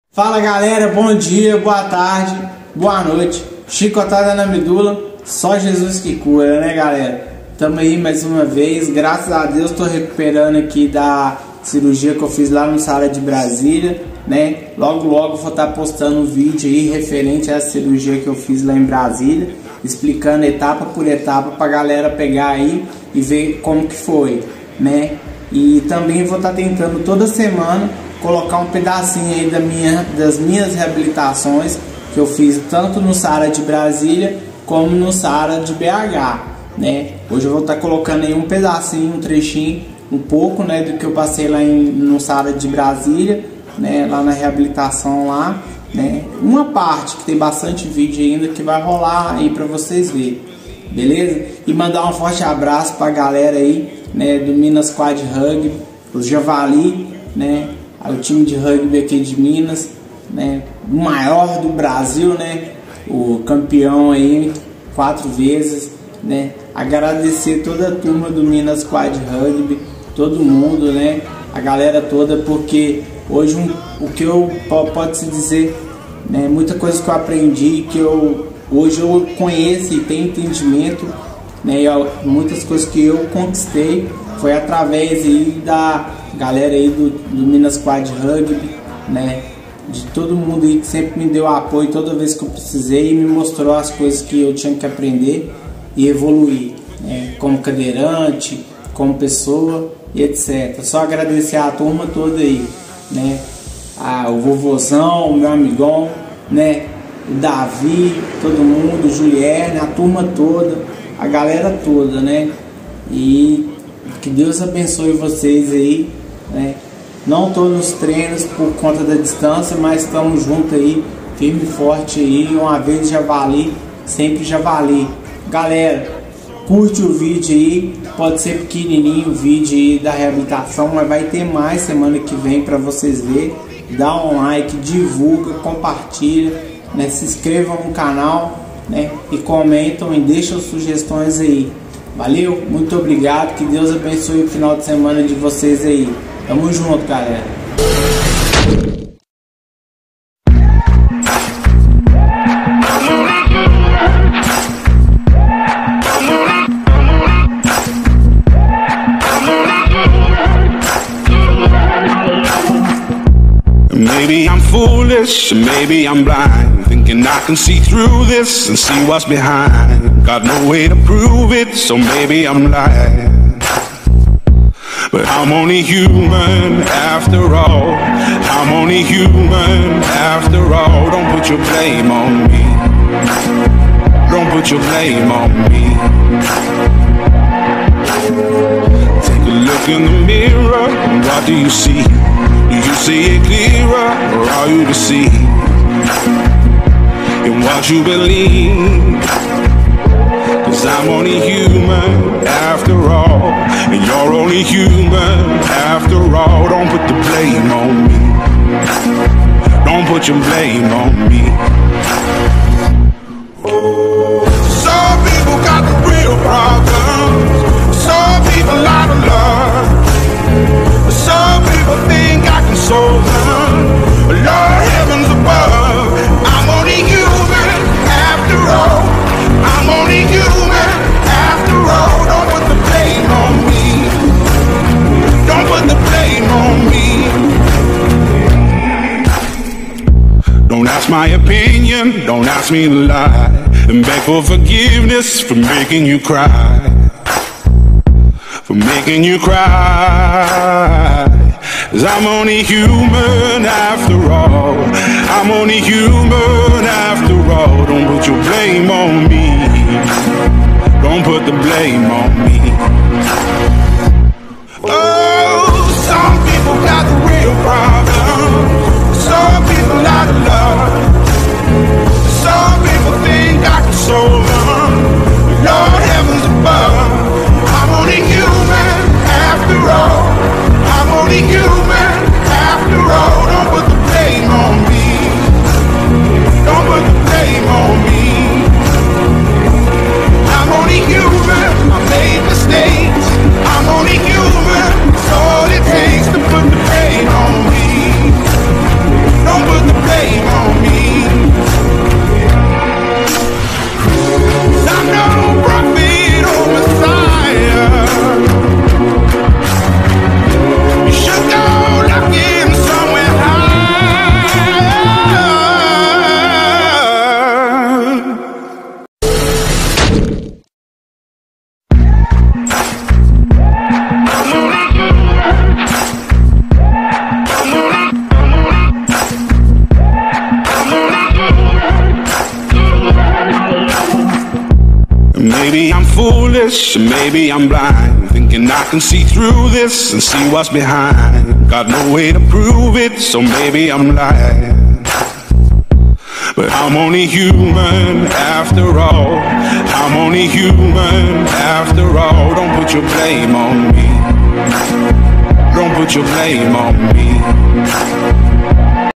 Sarah Kubitschek de Brasília-DF
Um pouco da reabilitação no Sarah Kubitschek de Brasília aonde eu tive a oportunidade de fazer duas reabilitação e também uma internação para uma cirurgia de transferência de nervos a onde irei colocar em breve os vídeos aqui no canal e acompanhe que terão muito mais vídeos para vocês, deixe nos comentários sugestões para vídeos.